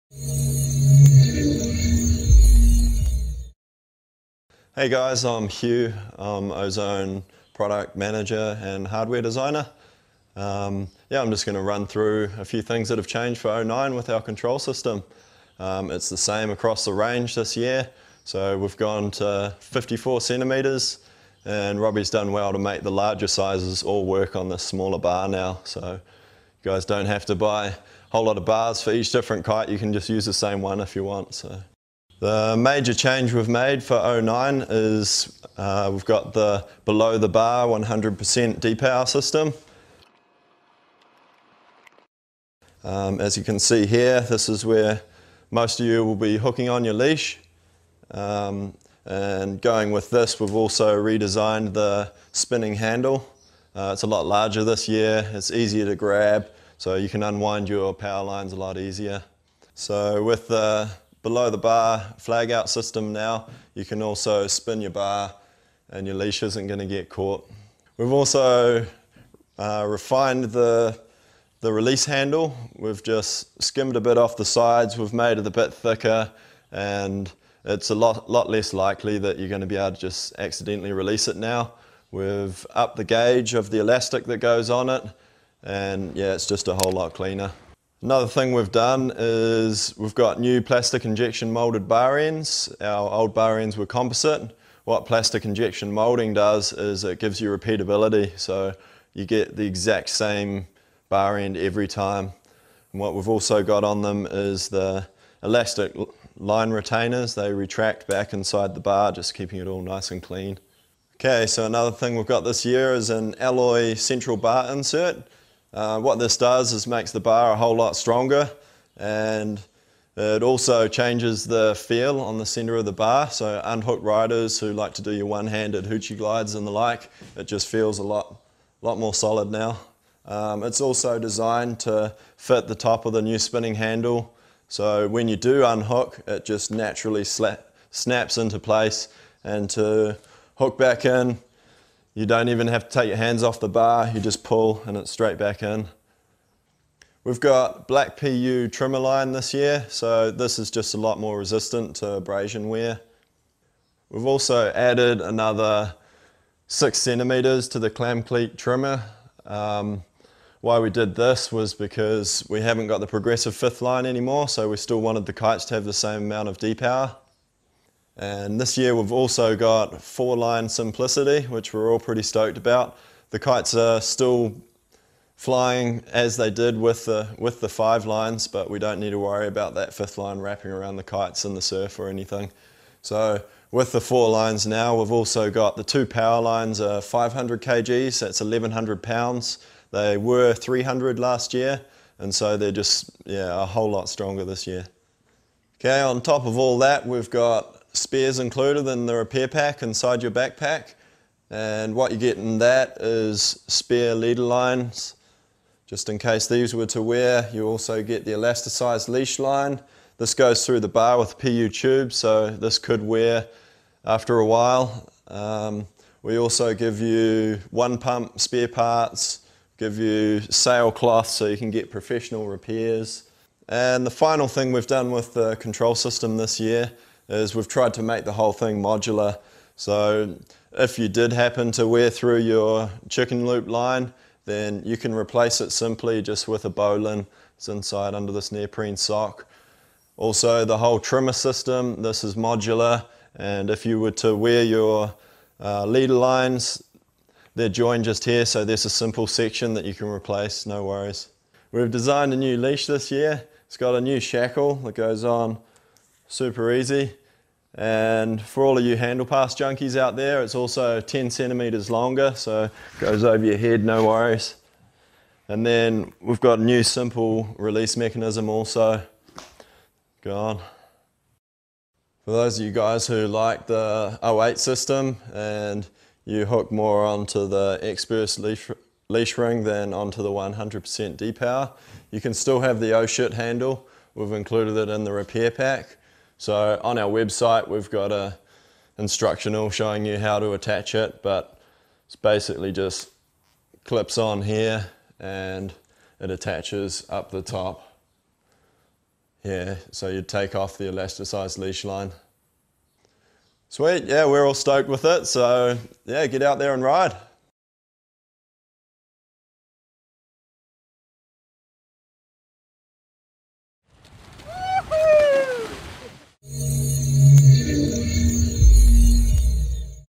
Ozone Instinct Kiteboarding Control System
Ergonomic bigger spinning handle  no more tangled power lines. One handed effortless & intuitive re-load for the D-Power-Loop. Large smooth release handle  easy to locate. Below bar safety system allows the bar to spin without twisting up your leash. Expert Leash attachment ring on Molded D-Power-loop for Experts to leash to for tangle-free un-hooked riding. Innovative high grade stainless Power Bracket, allowing for flawless flag-out safety system. D-Power trim strap which gives each kite a constant feel and direct handling throughout the trim range. Wide Mouth  D-Power Loop  Super easy for hooking in and out. Bar insert  For effortless unhooked re-loading. Swiveling D-Power Loop lock in finger  Easy to move out the way when riding unhooked

New 09 bar with moulded bar ends with elastic line fasteners to keep your lines tidy. Clearly colour coded bar ends. Foam leader line floats to protect your hands and to keep the bar floating. Back line Trimmers  adjust your back lines with ease and choose your preferred setting. Clamcleat Trimmer  Effortless to use for extra depower or when riding unhooked.